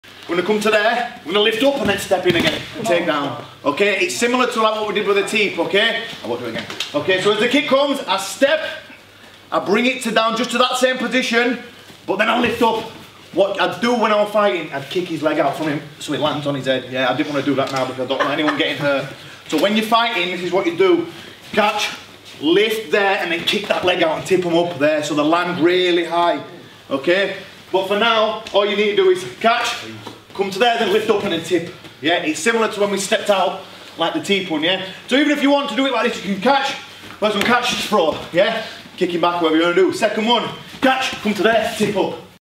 Killer Muay Thai Sweeps with Liam Harrison
Muay Thai sweep demonstrated by World Champion, ONE Championship signed fighter and internationally renowned fighter Liam Harrison.

For more in-depth full length instructional volumes on Muay Thai or other Martial Art disciplines,